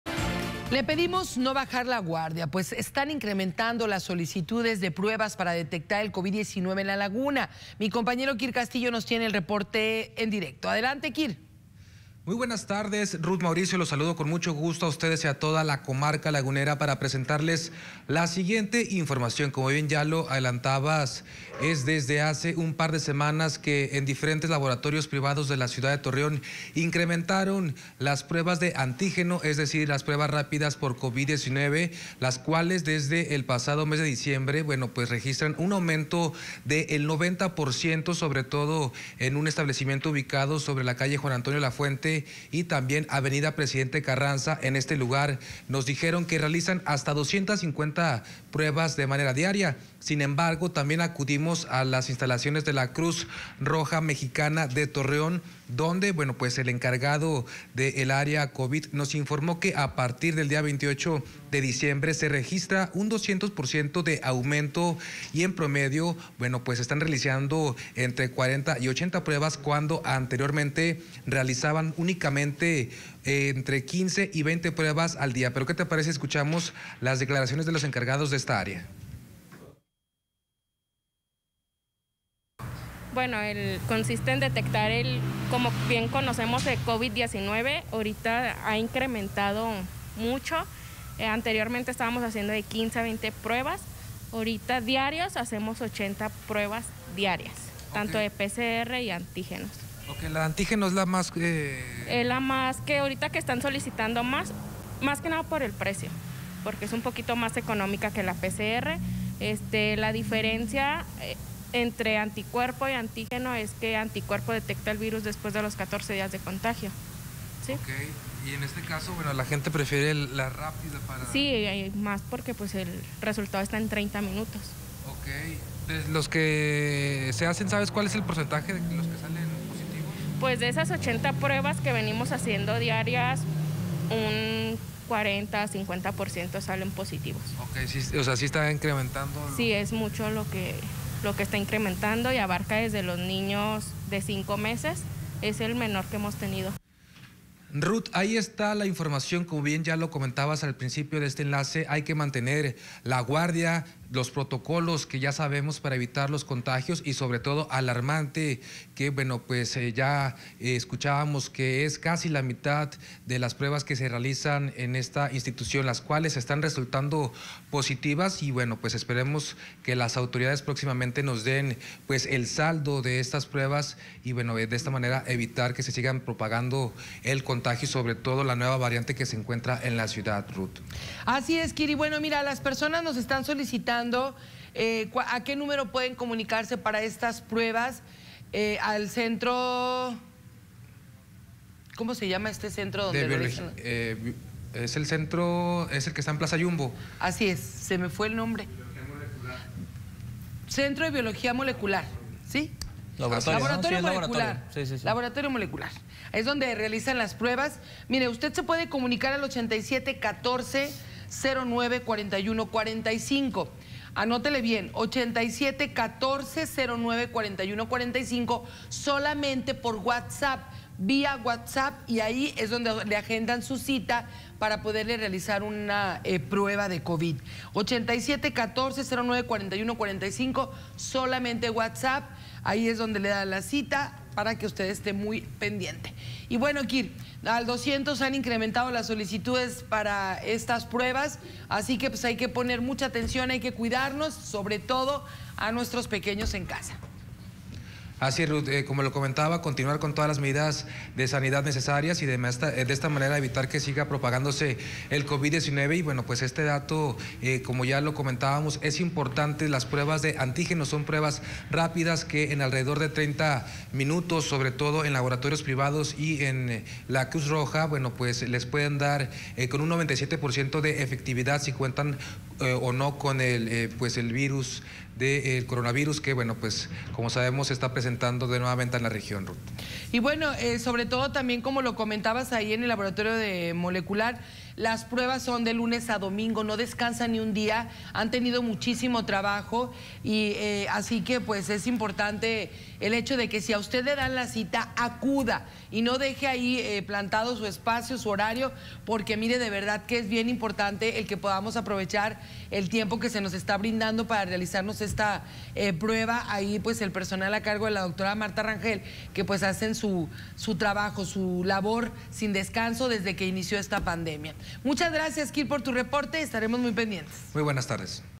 Incrementan pruebas por Covid 19
Incrementan en un 200% las solicitudes de pruebas rápidas y de PCR para detectar el virus de Covid 19 en la Laguna.